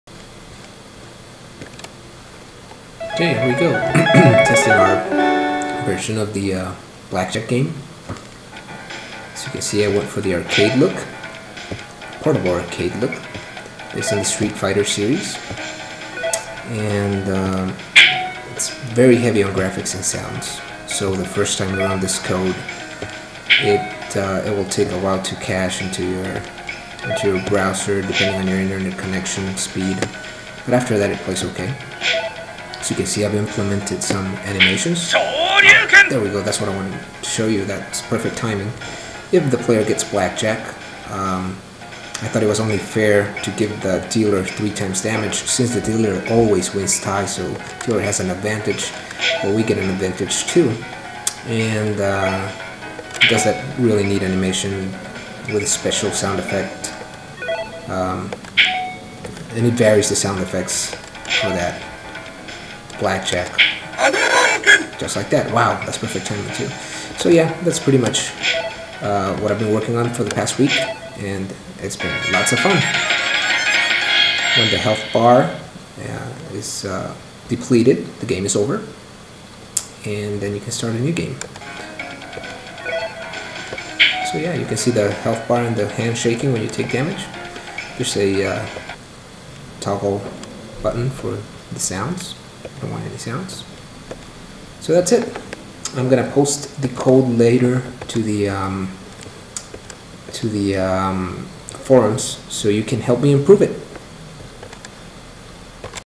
Street Fighter Blackjack in python
Project 6 for the Introduction to interactive programming in python course for coursera.